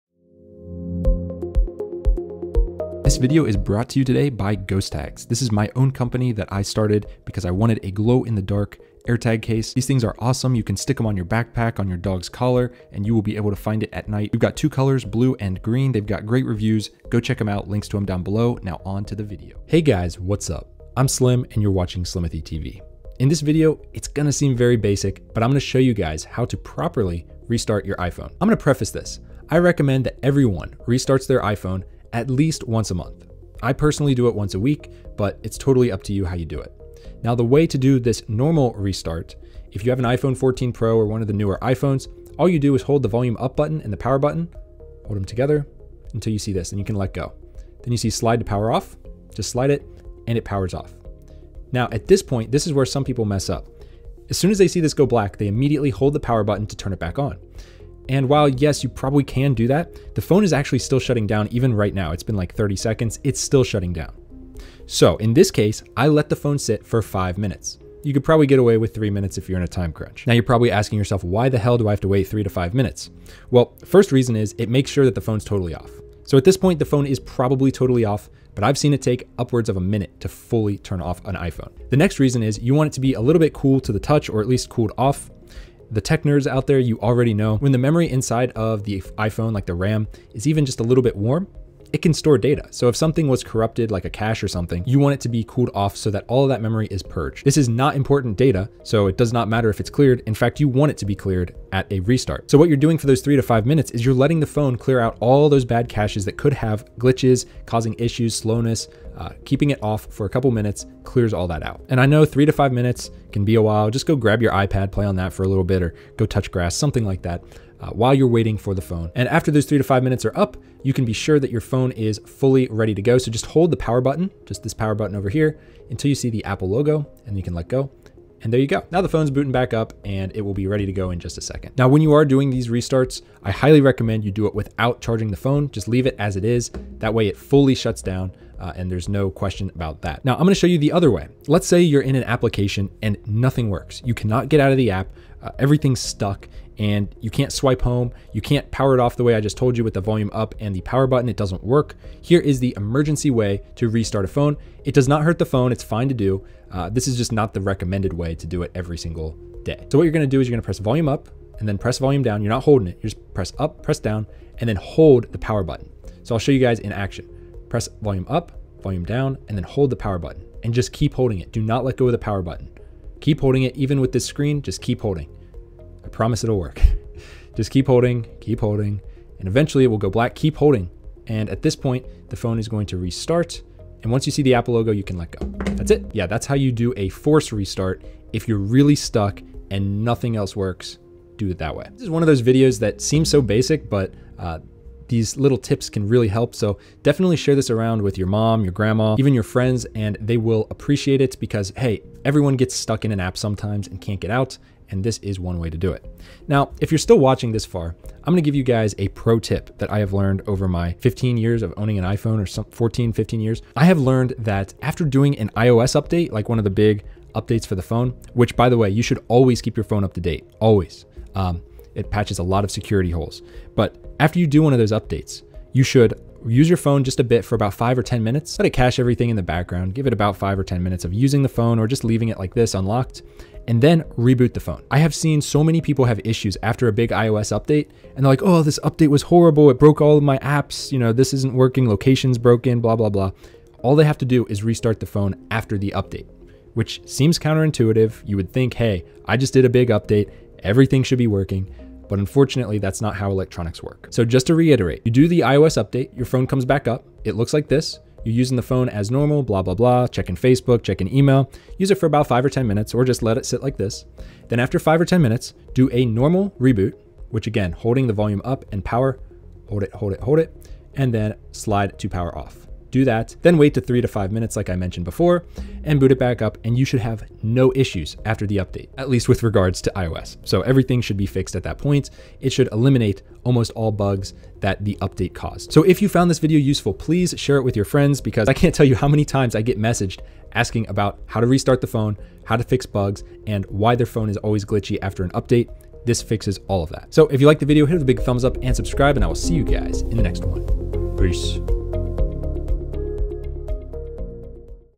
How to Restart the iPhone [The CORRECT Way to Reboot an iPhone!] 📱
In this video I show you how to restart and reboot your iPhone. This works for the newer model iPhones and shows you how to properly turn off your iPhone and how to power your iPhone down. Restarting your iPhone once a month can help your phone run faster, resolve bugs, and get rid of glitches that sometimes occur on an iPhone that's not rebooted as often. This video also shows you how to force restart the iPhone.

Link to purchase Blue Glow Ghostags
Link to purchase Green Glow Ghostags